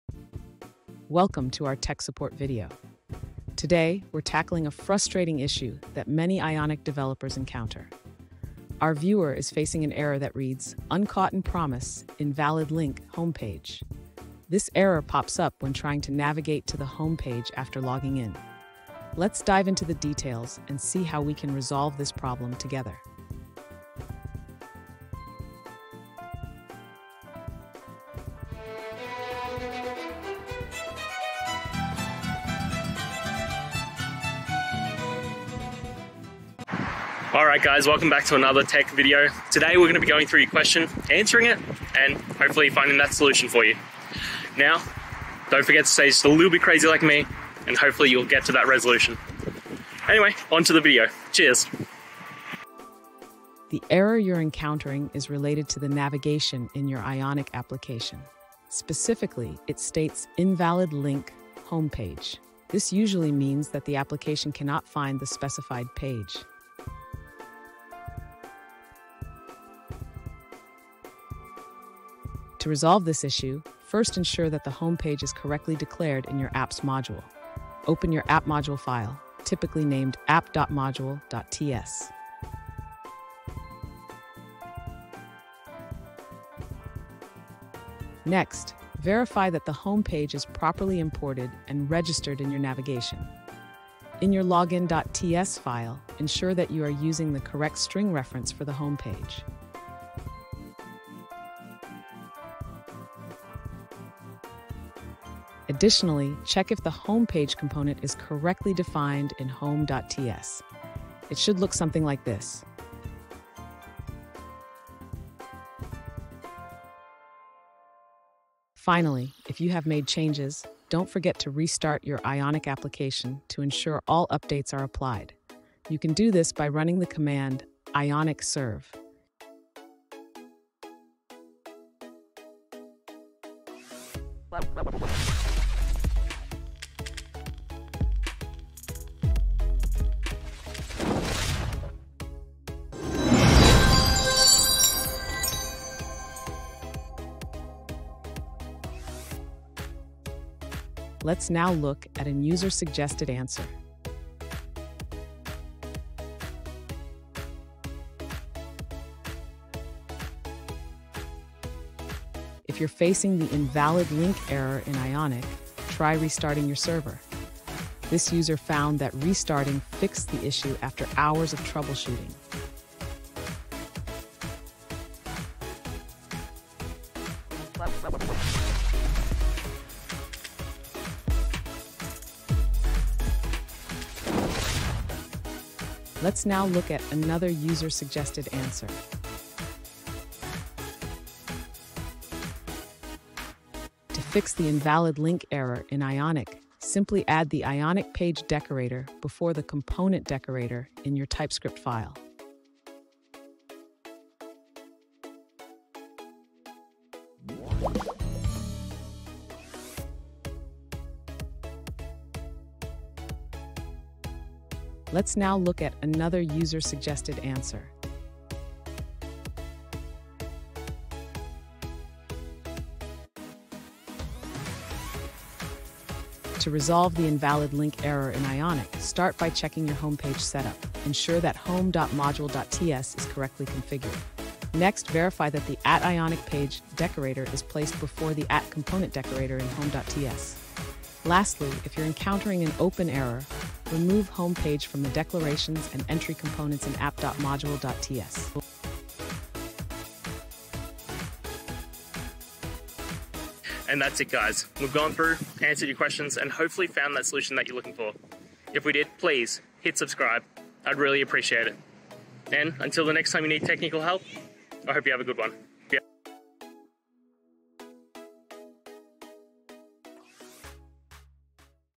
Ionic Uncaught (in promise): Fixing Invalid Link Errors in Your App
In this video, we dive into a common issue faced by developers using Ionic: the dreaded "Uncaught (in promise): Invalid Link" error. Whether you're building a mobile app or a web application, these errors can disrupt your workflow and frustrate users. Join us as we explore the causes behind this problem and provide step-by-step solutions to help you fix these invalid link errors, ensuring a smoother experience for both you and your app users.

Today's Topic: Ionic Uncaught (in promise): Fixing Invalid Link Errors in Your App

Related to: ionic, uncaught(inpromise),